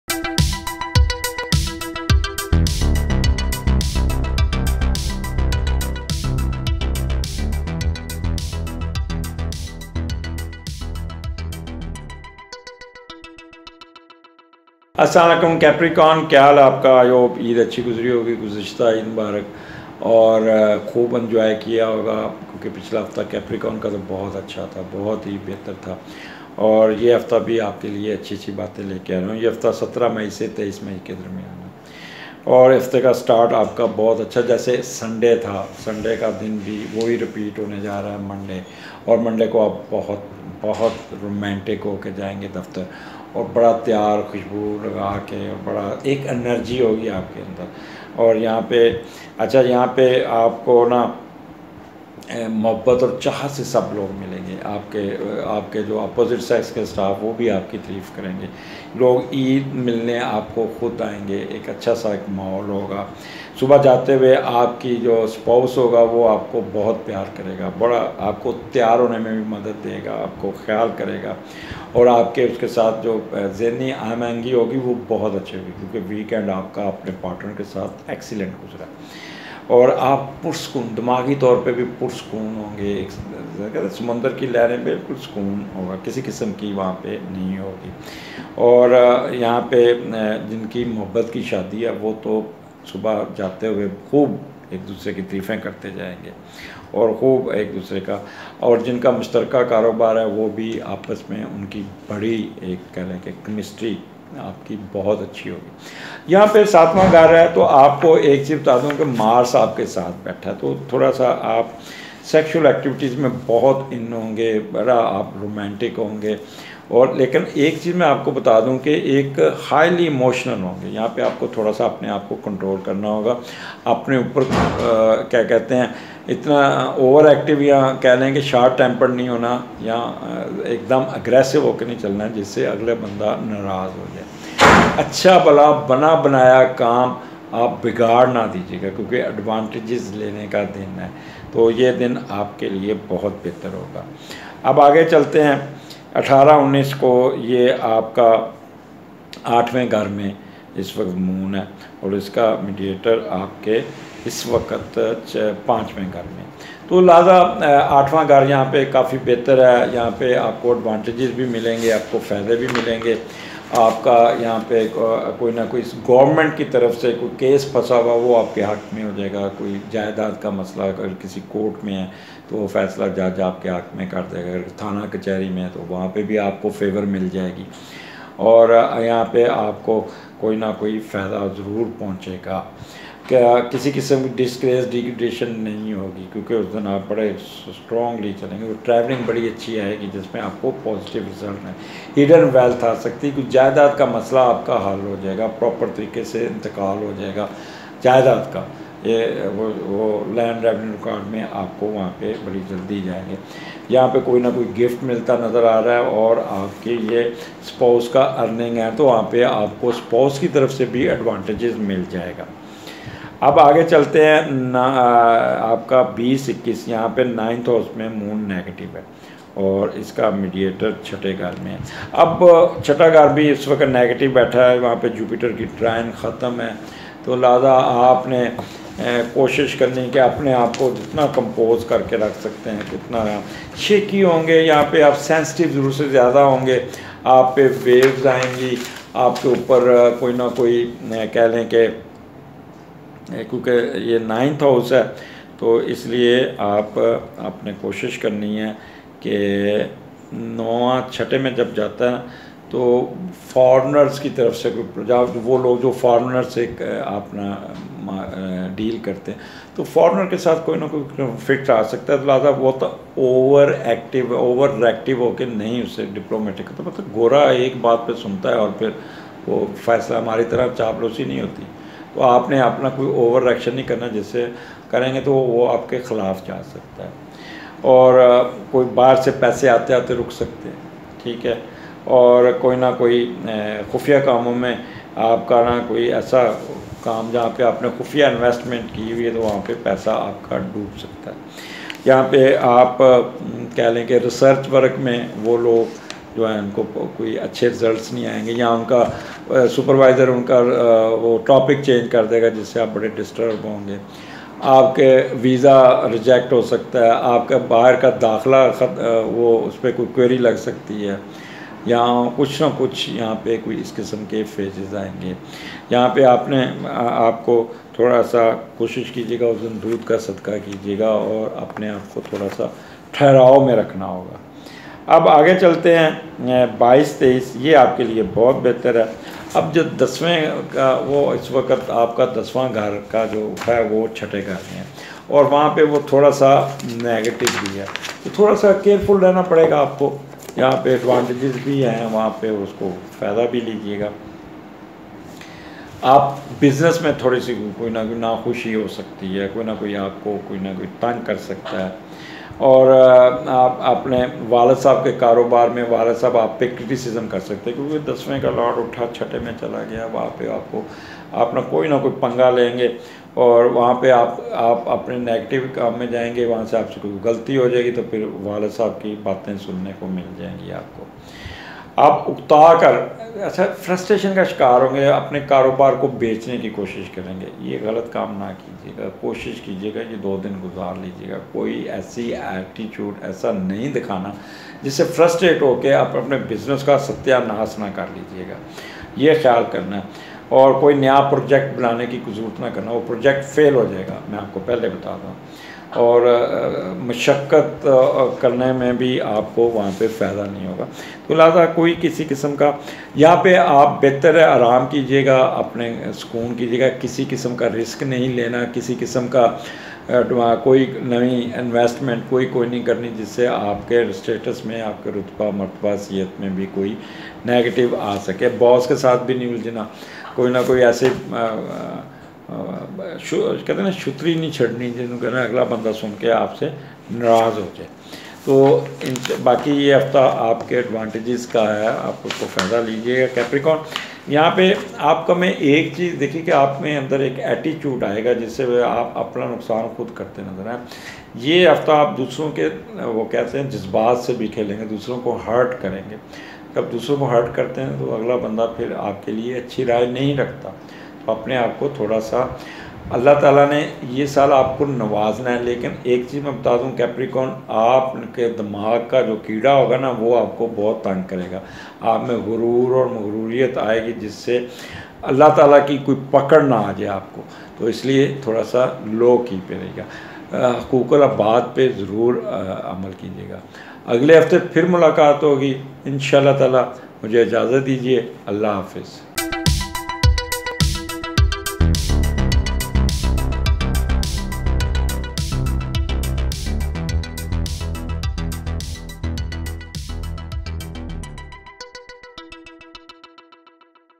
Carpicorn Weekly Horoscope 17 May to 23 May 2021 Syed M Ajmal Rahim
Weekly Horoscope by Syed Muhammad Ajmal Rahim from 17 May  to 23 May 2021  for Carpicorn.
Ajmal Rahim
Weekly Horoscope